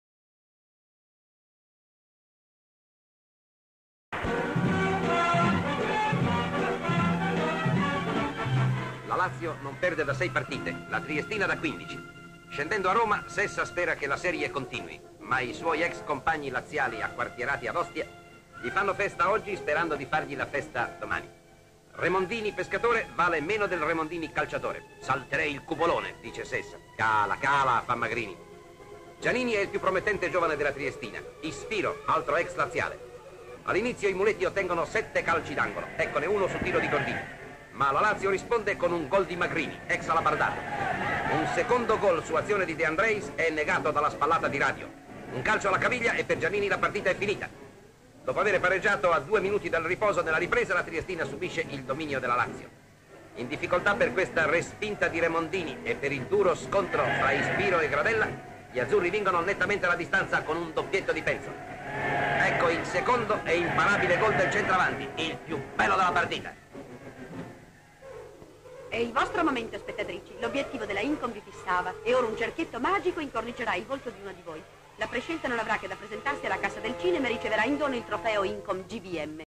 Campionato di calcio: girone di ritorno X giornata
Campionati di calcio: Lazio Triestina 3-1
Descrizione sequenze:i giocatori scendono alla stazione ; Sessa con i suoi amici ad Ostia ; Remondini pesca ; squadre in campo ; Gianini steso a terra ; giocatore segna una rete ; pubblico tifa sugli spalti ; focalizzazione con il "cerchietto magico" di una ragazza tra il pubblico ;